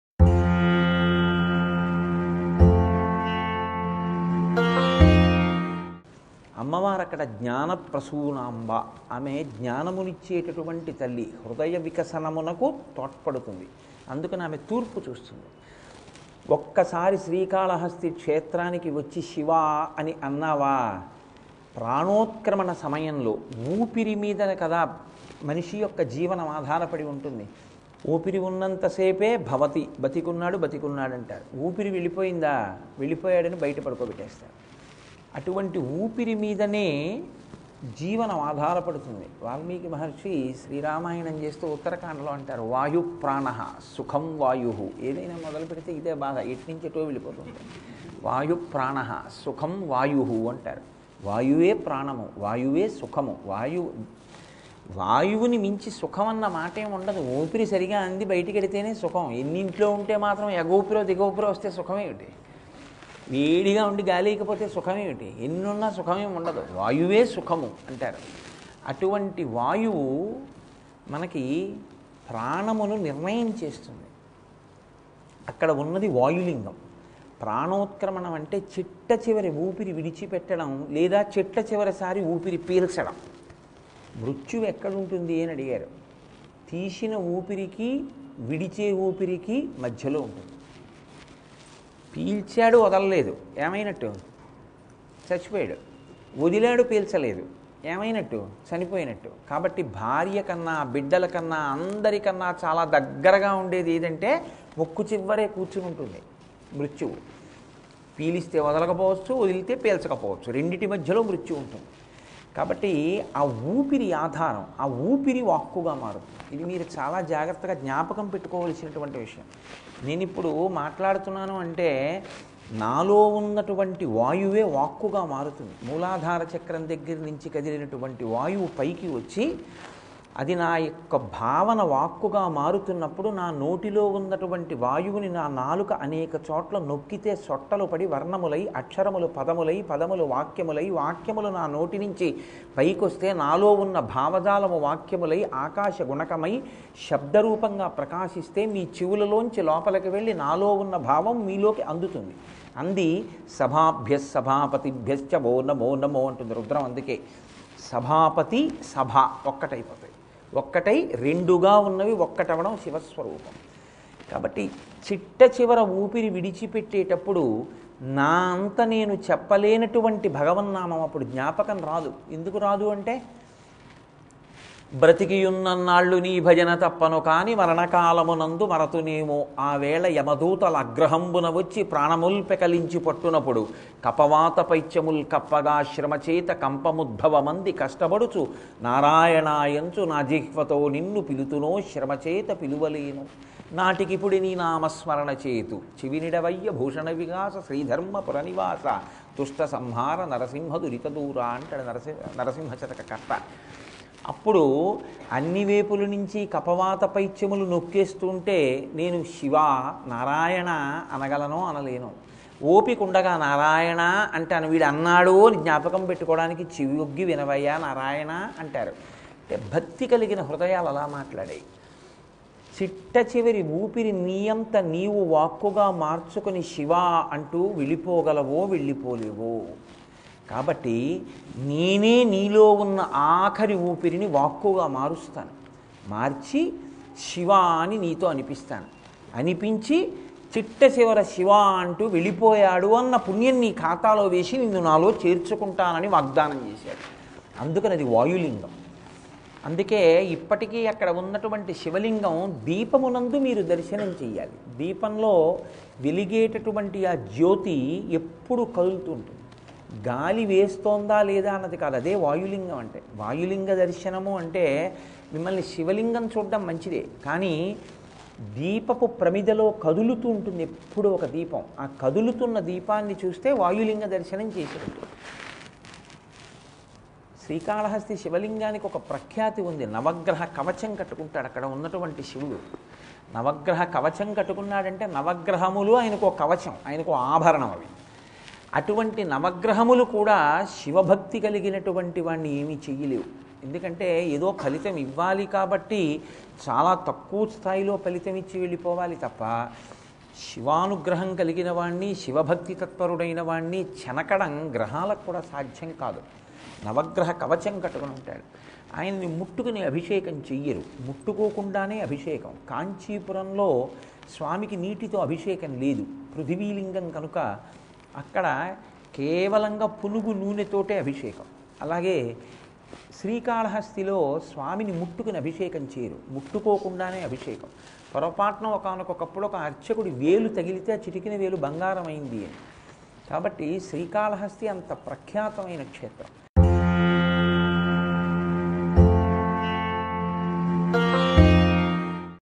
Sri kalahasthi | Dont touch the mula virat in kalahasthi
Alaya Rahasyalu Sri kalahasthi

Srikalahasti Temple is located in the town of Srikalahasti, in the state of Andhra Pradesh, India. It is one of the most famous Shiva temples in South India, and is said to be the site where Kannappa was ready to offer both his eyes to cover blood flowing from the Siva linga before the Siva stopped him and granted him mukti.